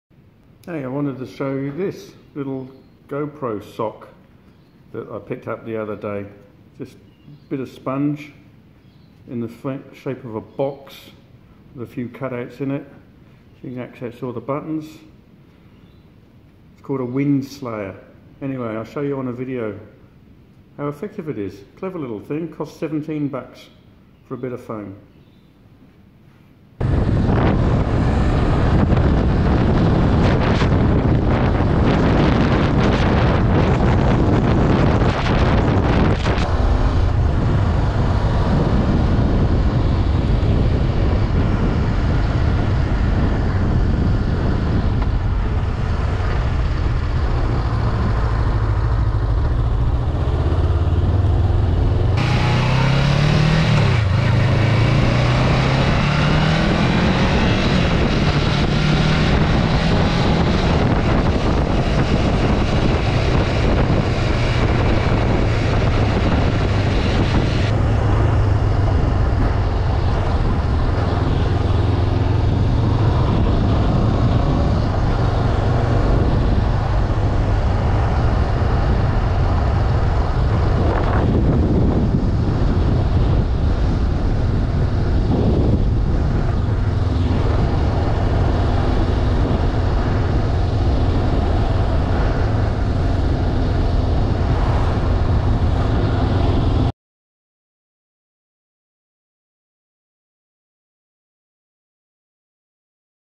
GoPro Windslayer Noise Reduction Demo
Demo with/without my Windslayer noise reduction foam case for GoPro.